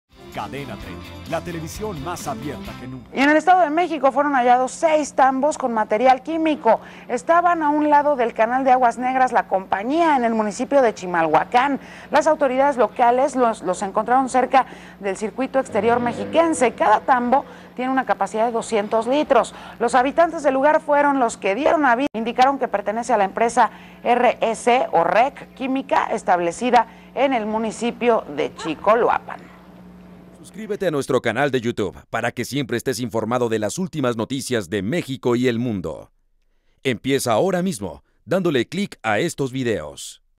Hallan tambos con material tóxico en Chimalhuacán
29 agosto 2014

En el Estado de México son encontrados 6 tambos con material químico a un lado del canal de aguas negras “La compañía” en Chimalhuacán.